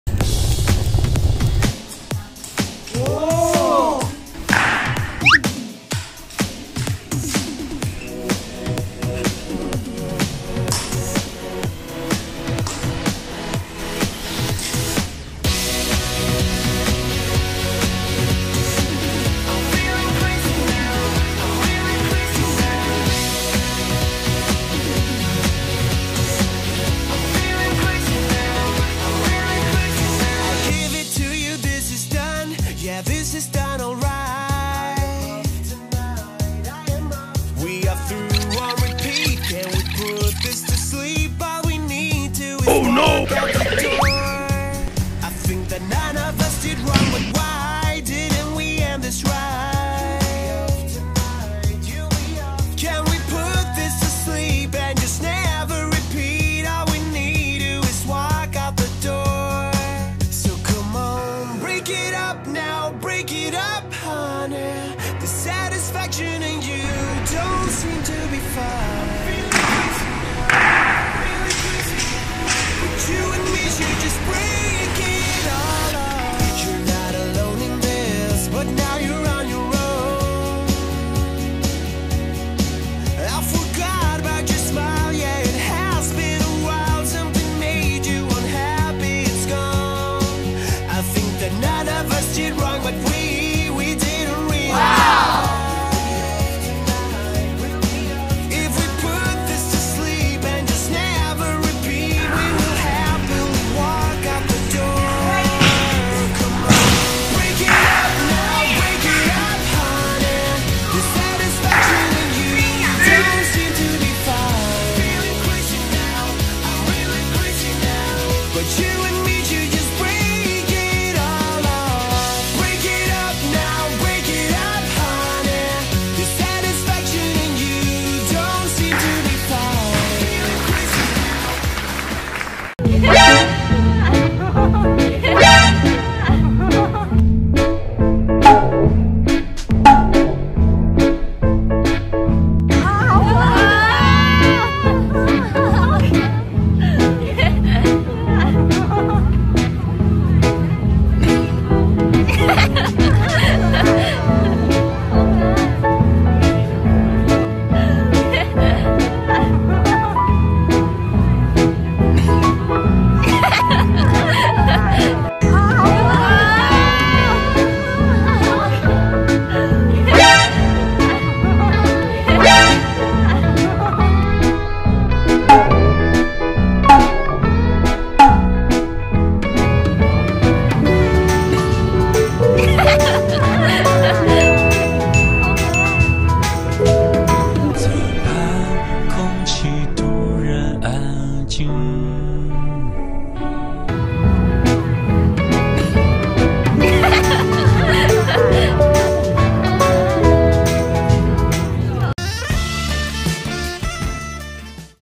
4歲娃全裝扮禮兵 完美演出操槍動作 網友讚：最萌小小兵有前途 | 萌娃
推薦觀看...

☑影片授權：FB 迷你禮兵孫孫